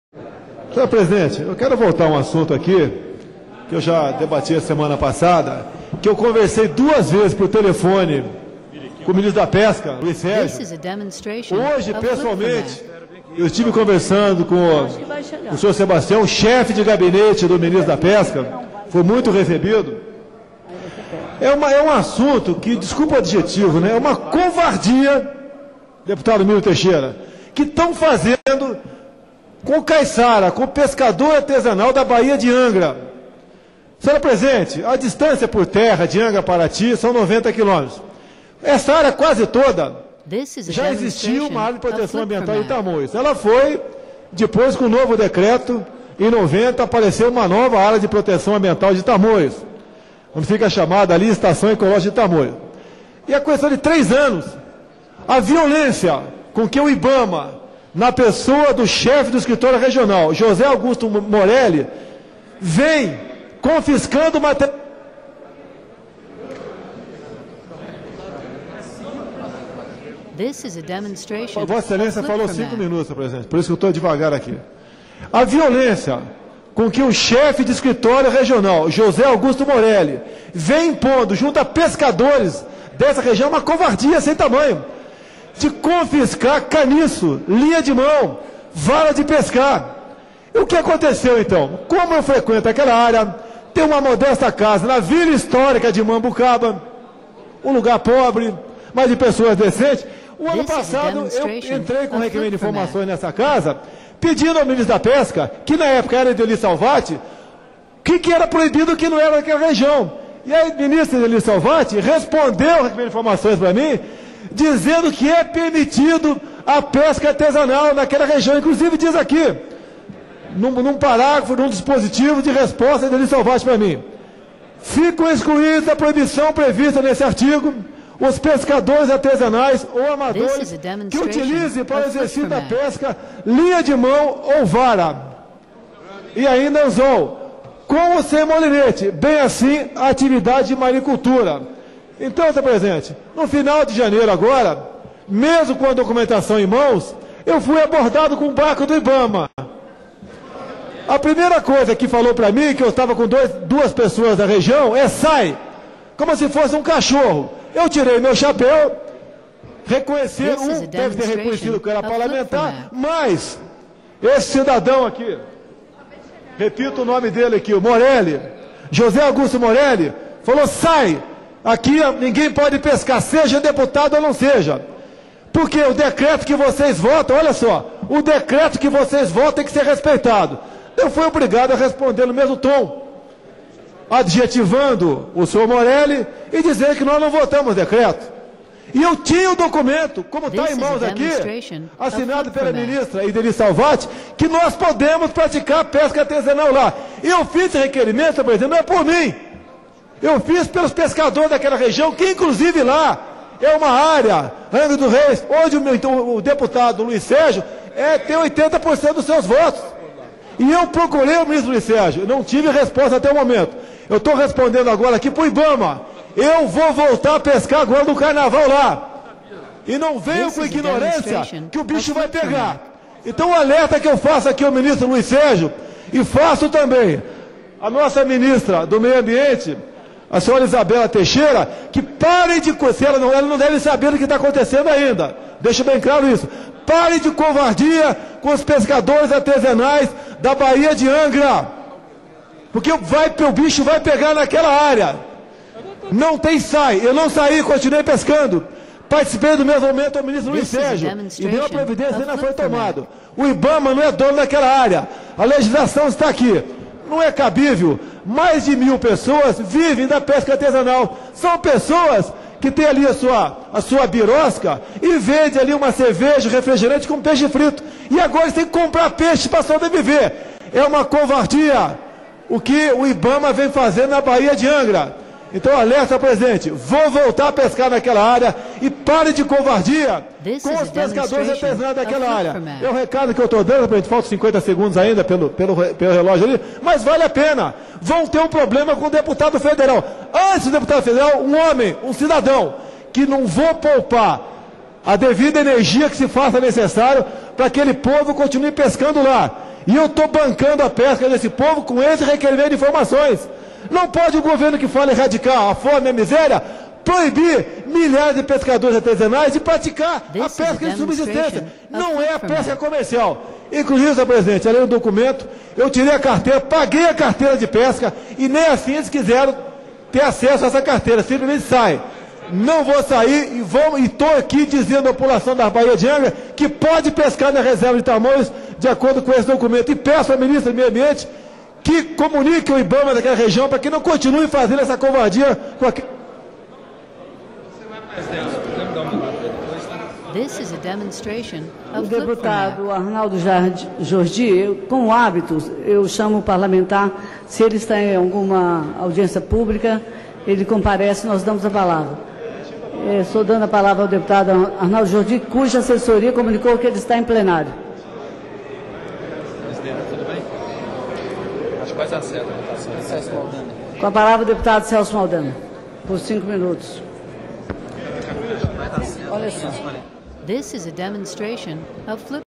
BOLSONARO DEFENDE PESCADORES ARTESANAIS EM ANGRA DOS REIS/RJ.
O Deputado Federal Jair Bolsonaro (PP-RJ) sai em defesa dos pescadores artesanais da baia de Angra dos Reis, denunciando abusos cometidos por fiscais do IBAMA daquela região. Os quais deveriam combater a pesca de arrasto e outras práticas muito mais nocivas e realmente proibidas por lei quando o assunto é pesca. Na certeza de estar colaborando com a sobrevivência daqueles humildes cidadãos que se alimentam da pesca de subsistência, Bolsonaro se pôe à disposição e desafia a continuidade das arbitrariedades cometidas por alguns fiscais daquele local. Avante Deputado, Nunca se cale na defesa do Justo. Selva!!!